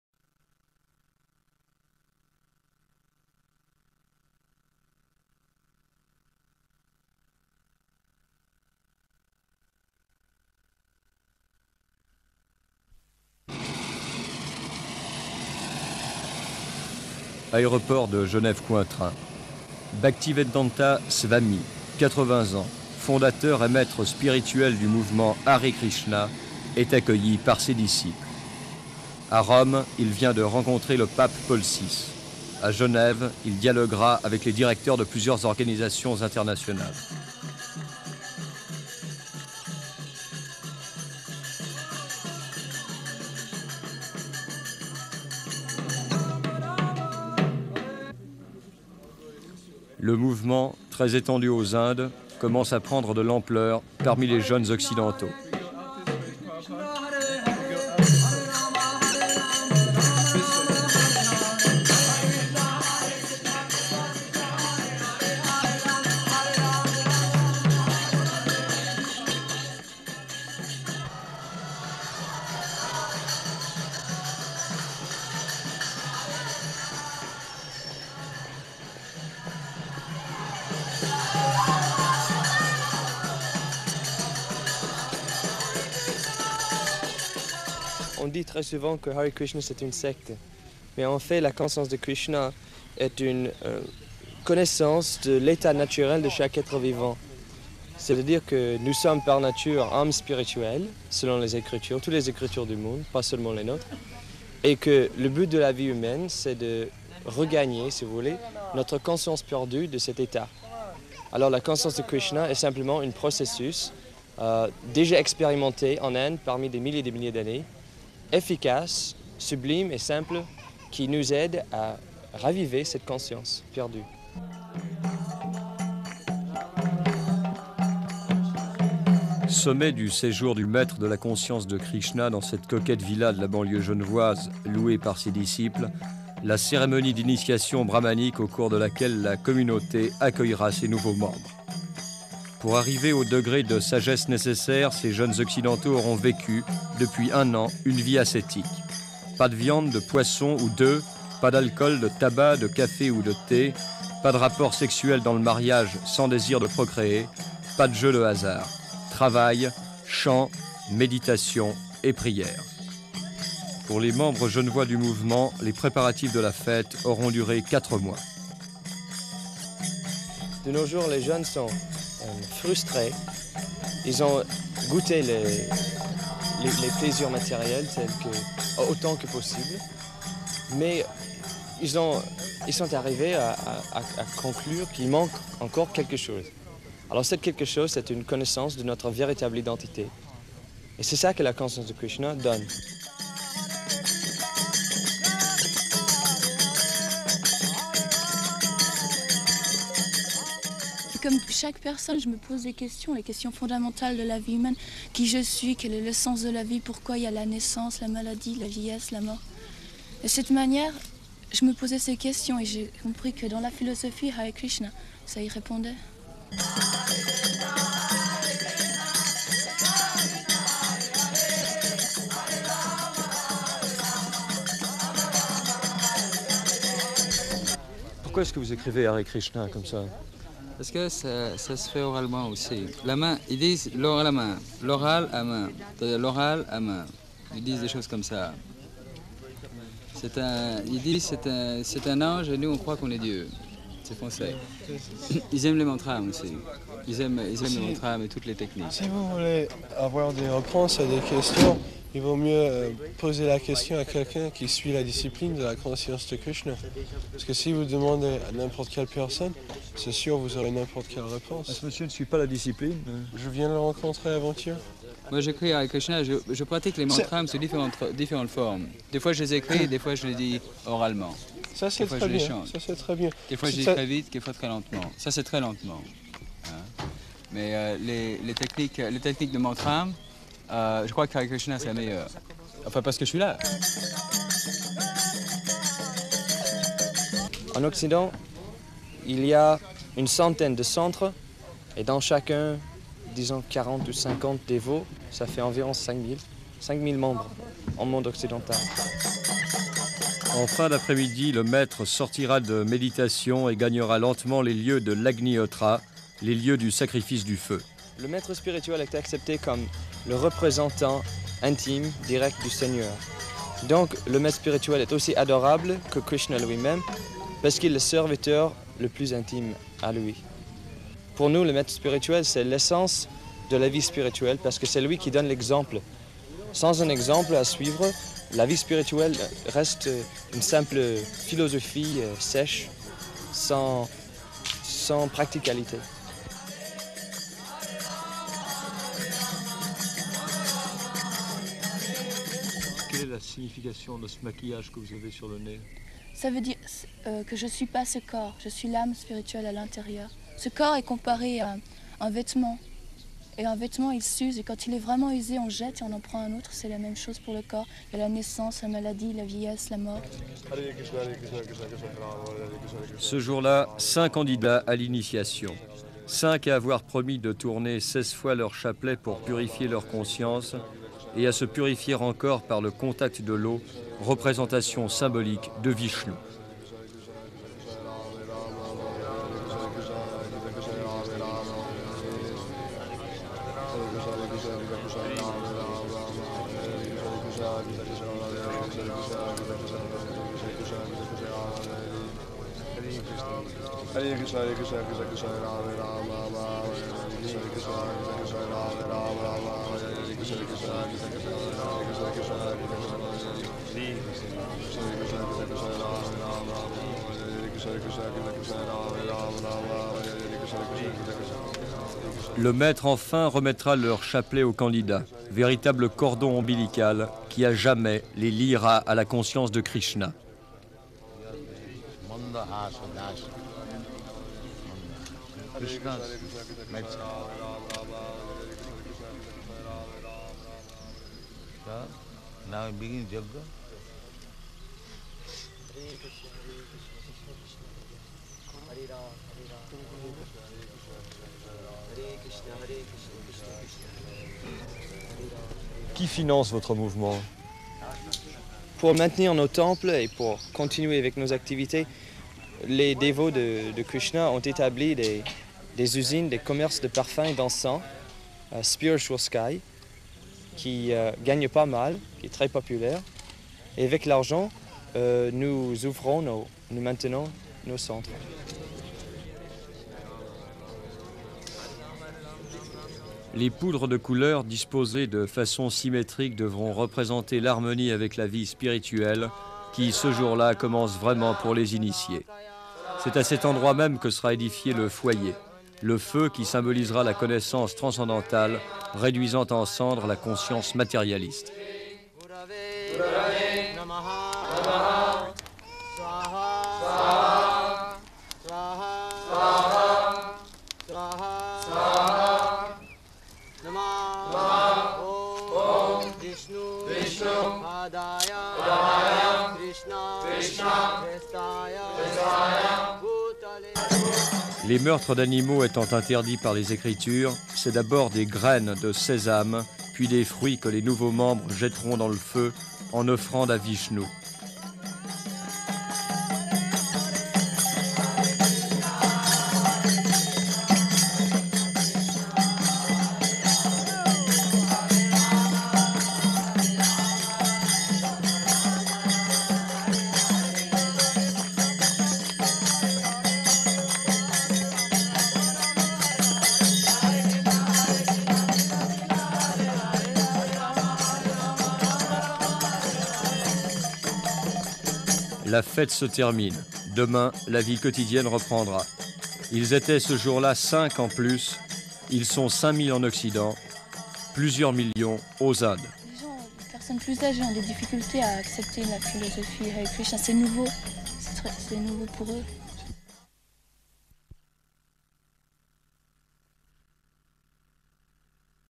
Srila Prabhupada en Suisse
Rare documentaire français sur Srila Prabhupada à Genève en 1974 pour donner des initiations à ses disciples, incluant Indradyumna Swami. (French)